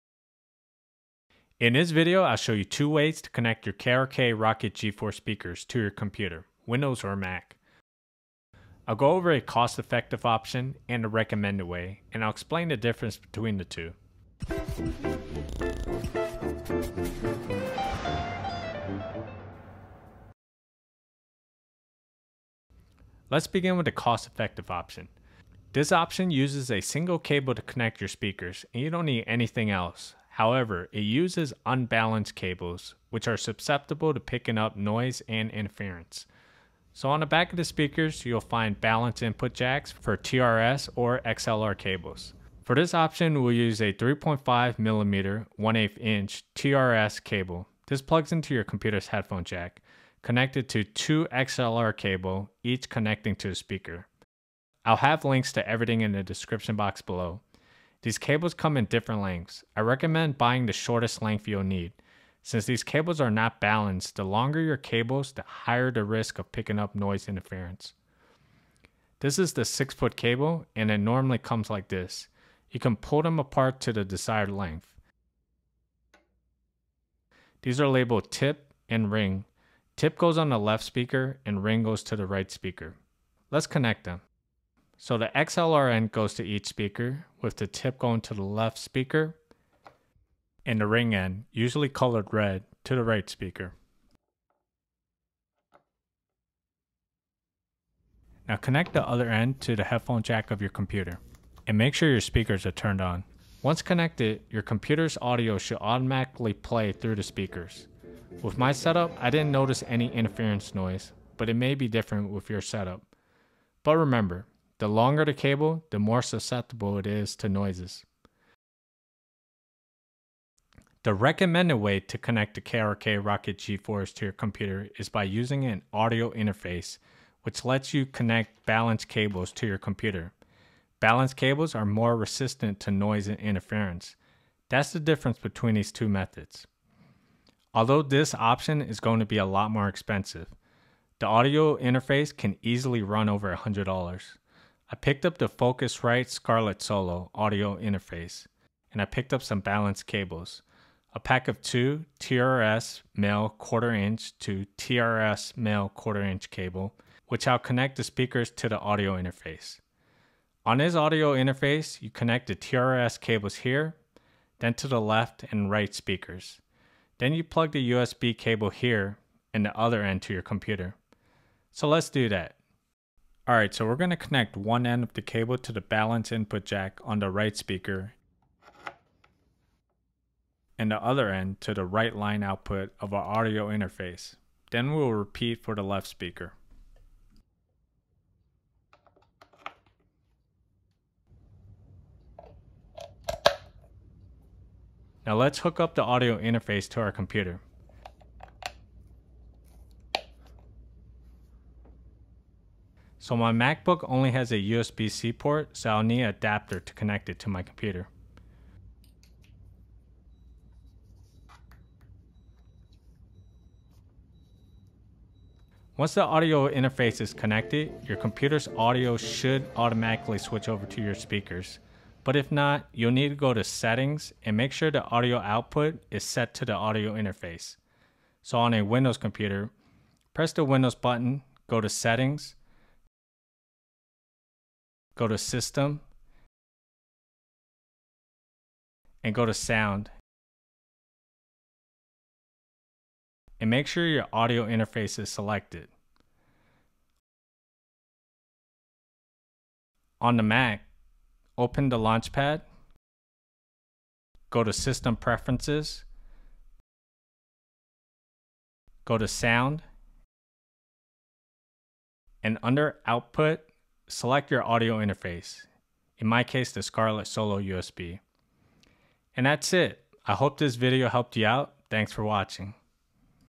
How-to Connect ROKIT KRK G4 Speakers to Computer
Learn how to connect your ROKIT KRK G4 speakers to your desktop or laptop computer (Windows/Mac). We will show you the cheapest option and the recommended option. Links to product in video:
3.5mm to 2 XLR Cable (cheap option)
Focusrite Scarlett Solo (recommended option)
TRS to TRS Cable, 2-Pack (reocmmended option)
ROKIT KRK G4 Speakers